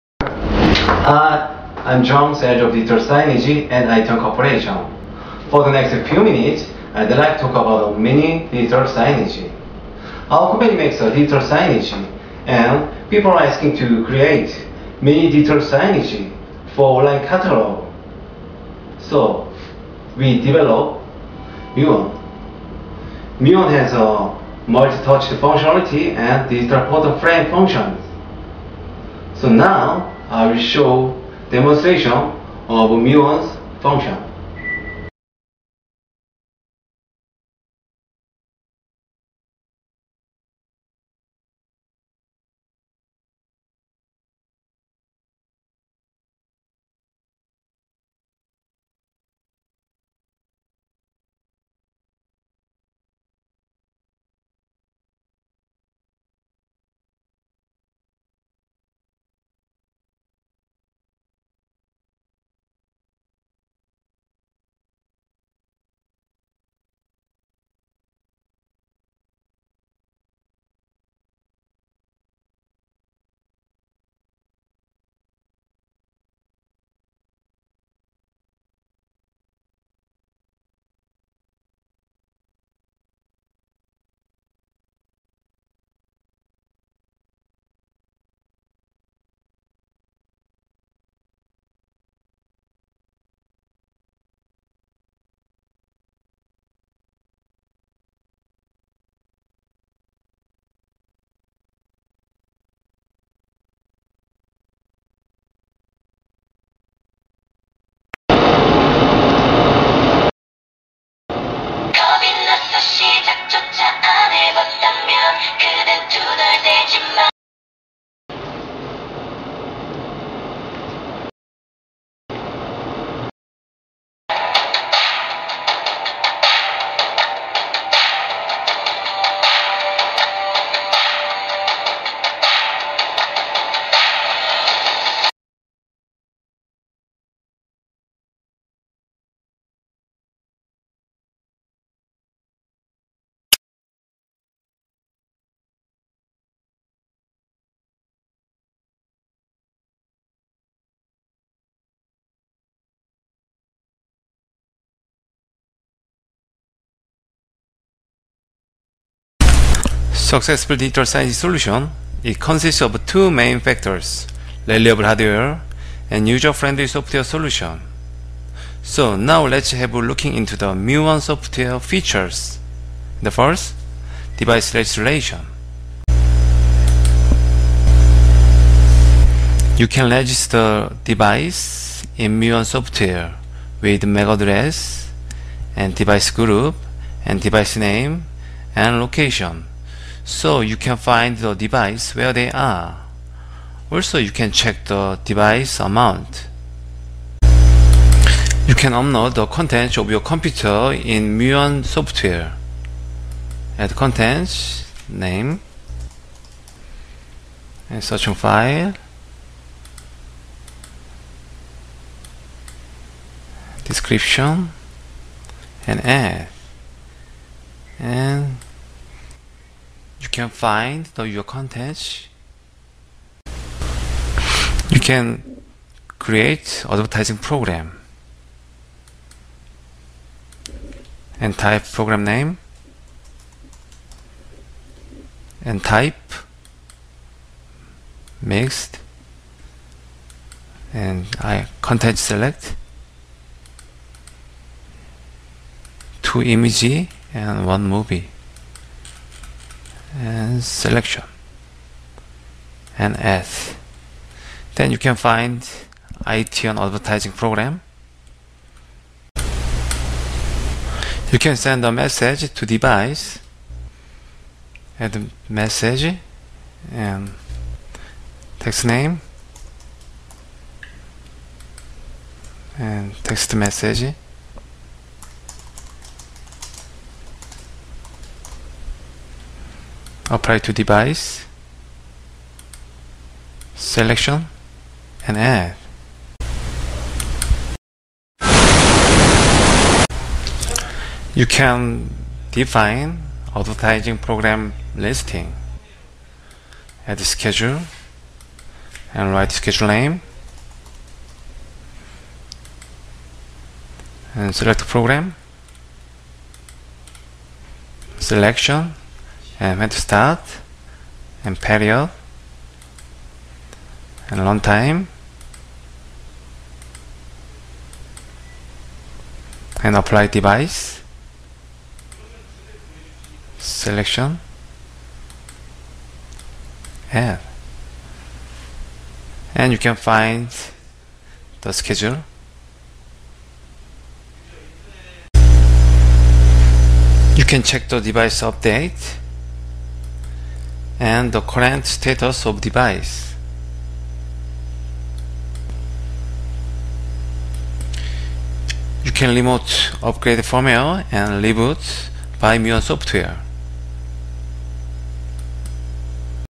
muon is advanced to networked based mini digital signage
- Contents delivered over Internet
- Play the contents at the predefined schedule
- Navigation by touch screen
- Digital photo frame